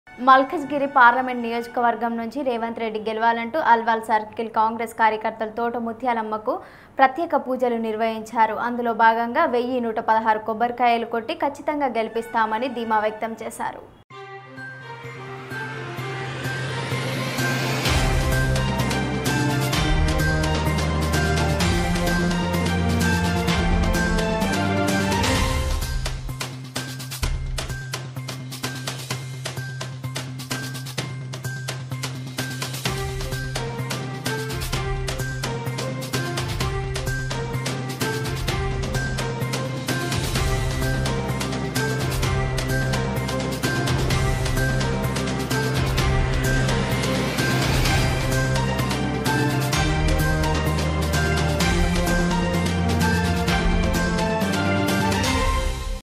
TS24 News మల్కాజిగిరి పార్లమెంట్ నియోజకవర్గం నుంచి రేవంత్ రెడ్డి గెలవాలని ముత్యాలమ్మ గుడిలో పూజలు
పార్లమెంట్ నియోజకవర్గం నుంచి రేవంత్ రెడ్డి గెలవాలని ముత్యాలమ్మ గుడిలో పూజలు